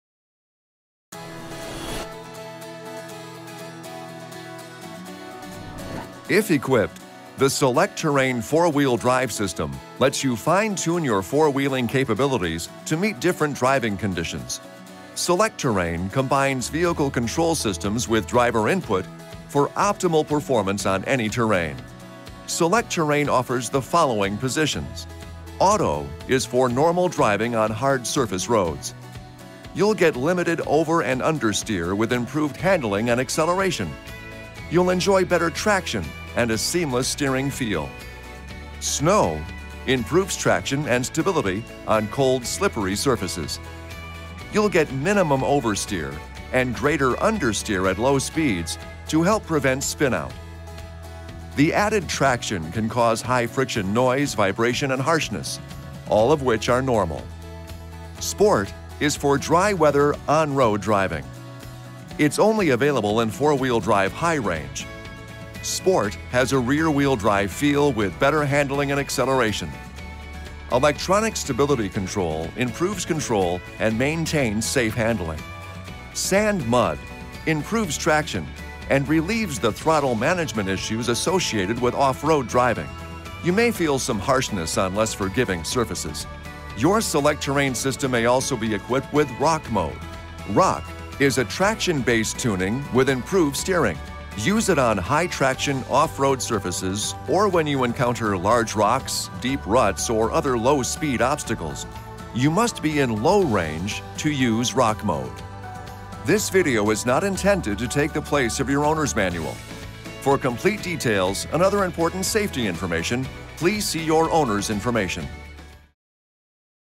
Selec-Terrain™-AWD for snow off road sand mud and rock 2018 Jeep Cherokee
If equipped, the 2018 Jeep Selec-Terrain™ 4X4 system allows you to place the transfer case in different automatic modes based on the terrain you are driving on. The auto mode allows you to choose the best setting for your driving conditions. In auto mode you may choose Snow, Off-road, sand, mud, or rock.